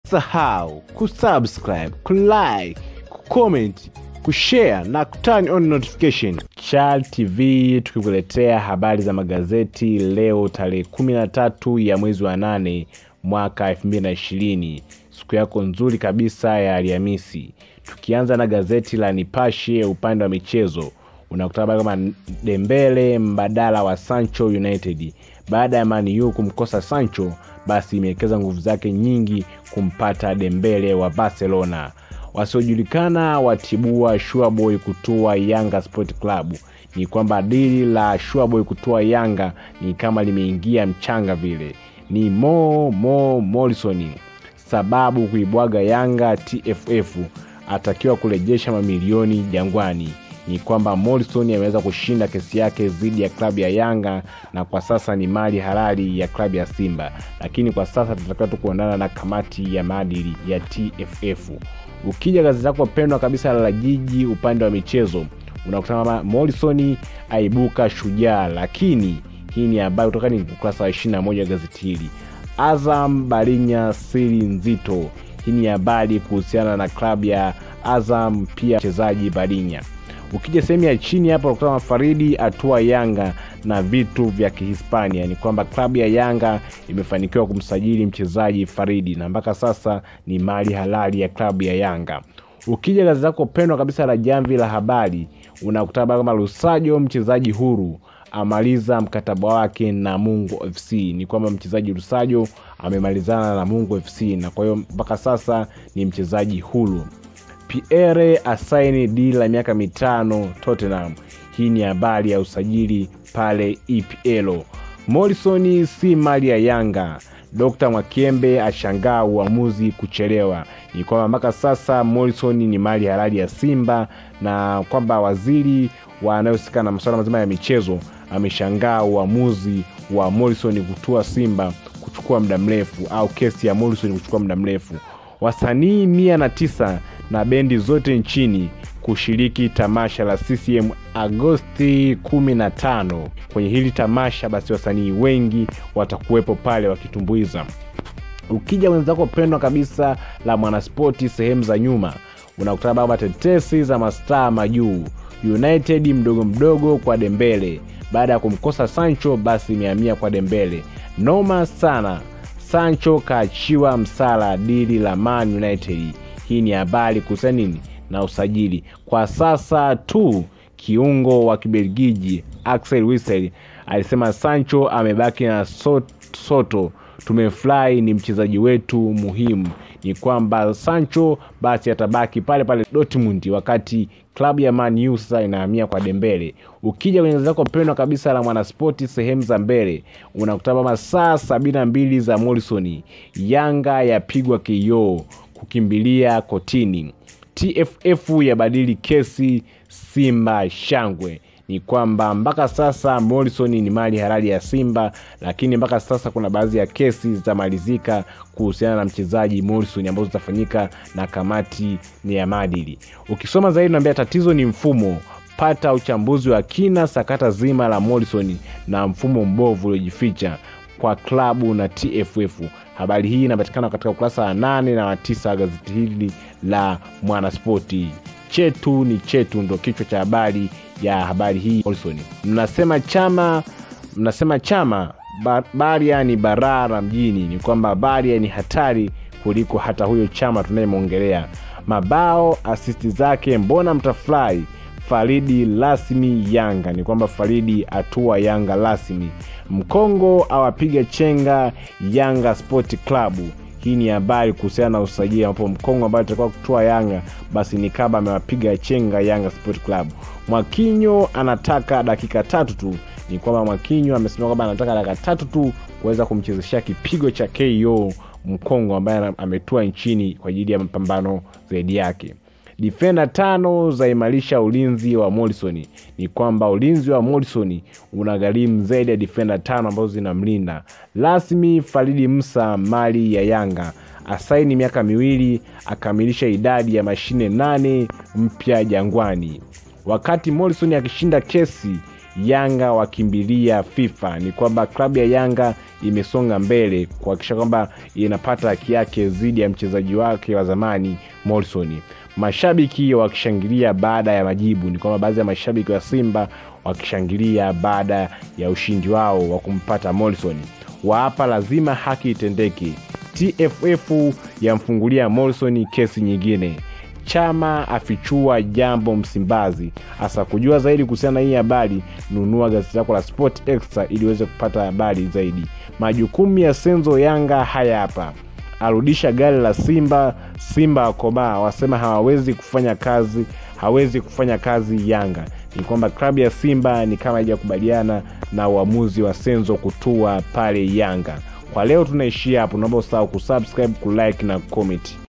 Michezo Magazetini 13/8/2020 Alhamisi/ Chama afichua jambo Simba/ Yanga wakimbilia FIFA, Morrison
Habari za magazeti leo tarehe 13/8/2020 siku ya Alhamisi